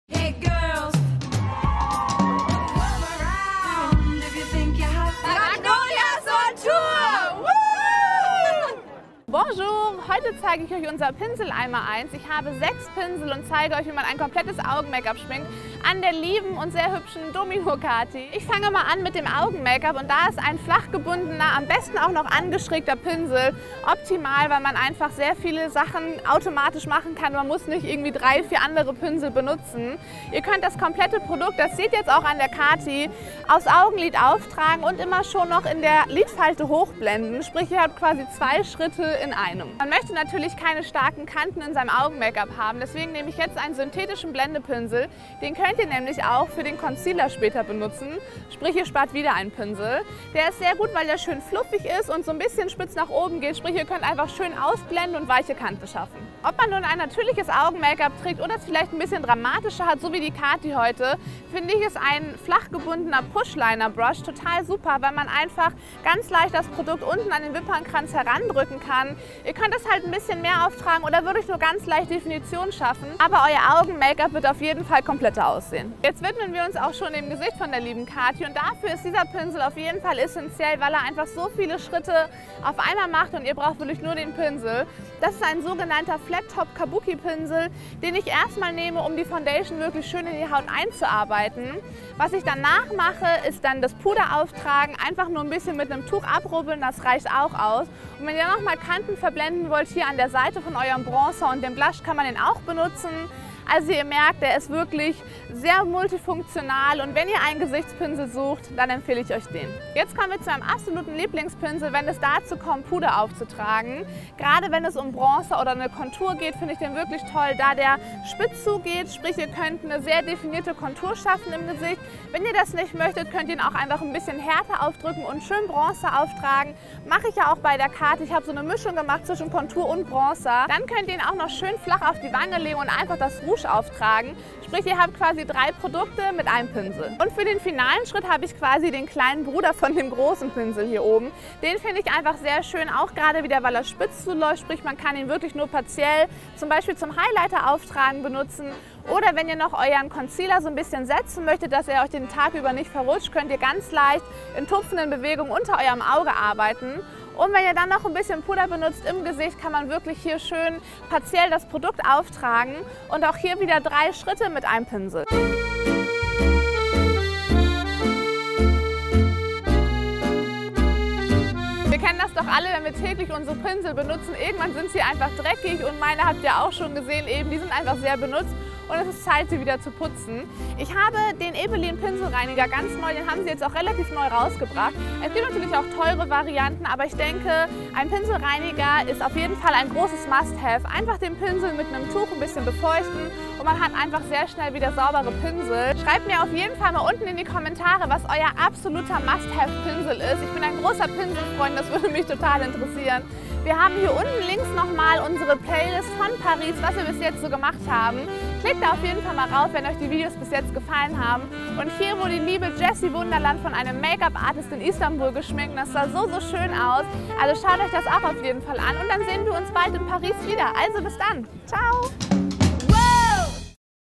Das Pinsel Einmaleins & Must Haves
Bonjour de la ville de l'amour! Heute zeigt euch Jamina an Kati welche Pinsel sie zum schminken verwendet und welche ihre persönlichen Must-Haves sind. Habt ihr einen ganz bestimmten Make-Up Pinsel, ohne den ihr nicht leben könnt?